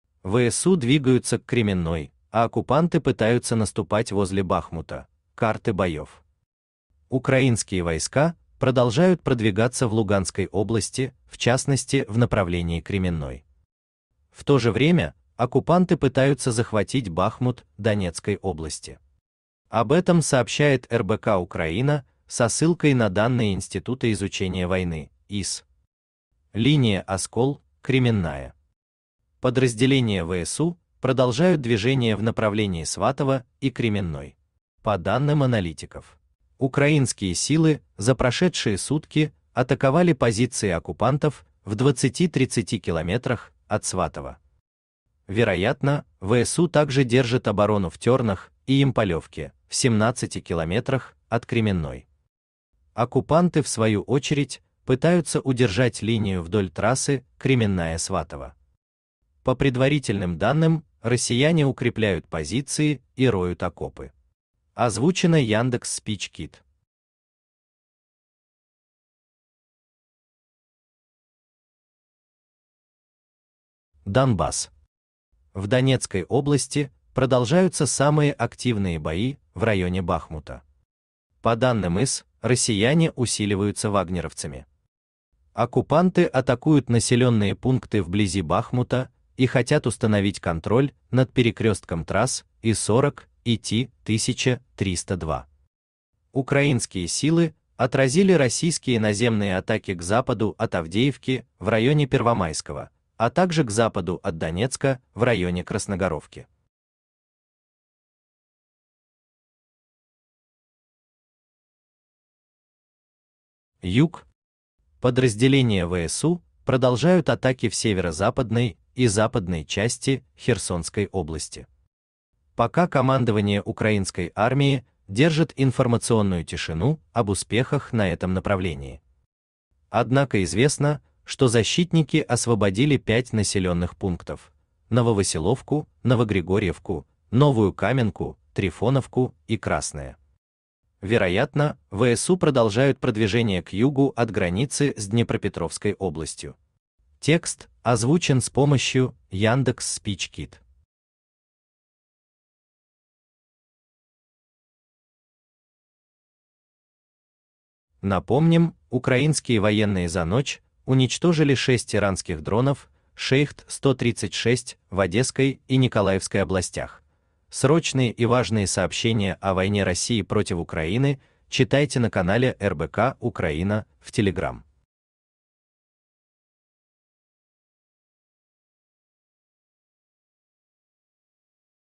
ВСУ двигаются к Кременной, а оккупанты пытаются наступать возле Бахмута: карты боев.
ВойнавУкраине ВойнаРоссиипротивУкраины Украинские войска продолжают продвигаться в Луганской области, в частности в направлении Кременной. В то же время оккупанты пытаются захватить Бахмут Донецкой области. Об этом сообщает РБК-Украина со ссылкой на данные Института изучения войны (ISW). Линия Оскол - Кременная.
Подразделения ВСУ продолжают движение в направлении Сватово и Кременной.  По данным аналитиков, украинские силы за прошедшие сутки атаковали позиции оккупантов в 20-30 км от Сватово. Вероятно, ВСУ также держат оборону в Тернах и